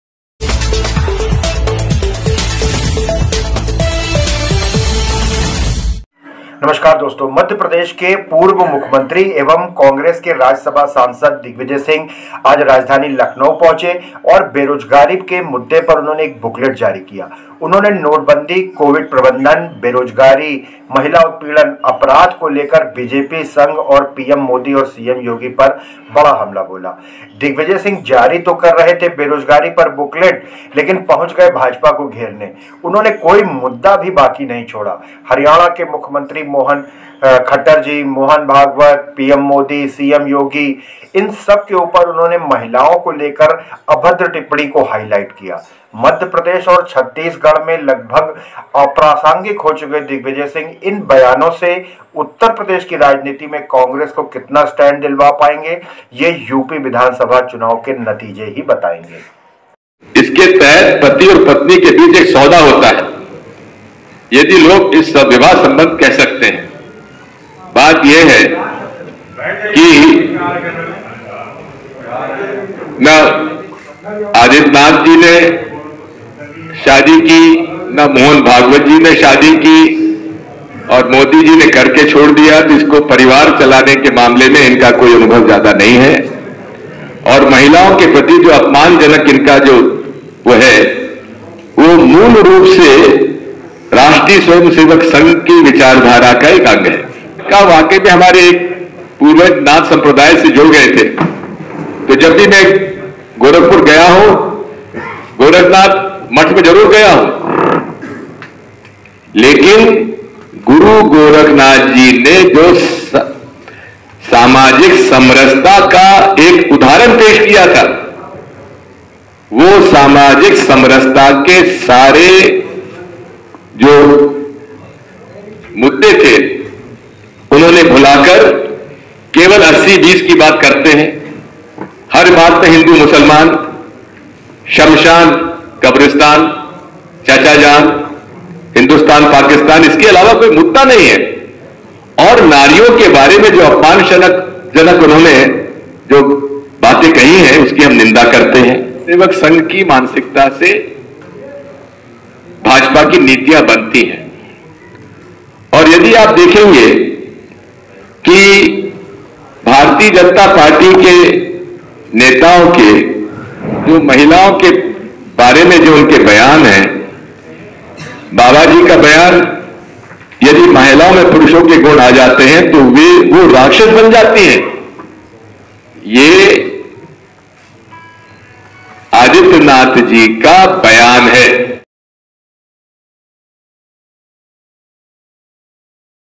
BJP, Rss, bhagwat ,PM modi और CM yogi महिला विरोधी - दिग्विजय सिंह
*national watch political news lucknow UP assembly elections 2022 latest updates congress MP former CM digvijay singh attack on mohan bhagavat PM modi BJP and up CM Yogi Adityanath for humiliation of women*

rss, bhagwat, BJP, modi और yogi महिला विरोधी - दिग्विजय सिंह

लखनऊ। मध्य प्रदेश के पूर्व मुख्यमंत्री एवं राज्ससभा सांसद दिग्विजय सिंह ने युवा बेरोजगारी के मुद्दे पर बुकलेट जारी किया। उन्होंने नोटबंदी, कोविड प्रबंधन, बेरोज़गारी, महिला उत्पीडन, अपराध को लेकर बीजेपी, संघ, पीएम मोदी और सीएम योगी पर हमला बोला। दिग्विजय सिंह जारी तो कर रहे थे बेरोज़गारी पर बुकलेट लेकिन भाजपा को घेरने में उन्होंने कोई मुद्दा बाकी नहीं छोड़ा, हरियाणा के मुख्यमंत्री, मोहन भागवत, पीएम मोदी और सीएम योगी पर महिलाओं को लेकर अभद्र टिप्पणियों को ही हाई लाइट किया। मध्यप्रदेश और छत्तीसगढ़ में लगभग अप्रासंगिक हो चुके दिग्विजय सिंह इन बयानों से उत्तर प्रदेश की राजनीति में कांग्रेस को कितना स्टैंड दिलवा पाएंगे ये यूपी विधानसभा चुनाव के नतीजे ही बताएंगे।

What do Bhagwat, Modi and Yogi women know - Digvijay Singh

Lucknow. Former Madhya Pradesh Chief Minister and Rajya Sabha MP Digvijay Singh released a booklet on the issue of youth unemployment. He attacked BJP, Sangh, PM Modi and CM Yogi for demonetisation, covid management, unemployment, women oppression, crime. Digvijay Singh was issuing a booklet on unemployment, but he did not leave any issue to surround the BJP, highlighting the indecent remarks about women on Haryana Chief Minister, Mohan Bhagwat, PM Modi and CM Yogi. Digvijay Singh, who has become almost irrelevant in Madhya Pradesh and Chhattisgarh, will tell the results of the UP assembly elections, how much stand the Congress will be able to get in the politics of Uttar Pradesh with these statements.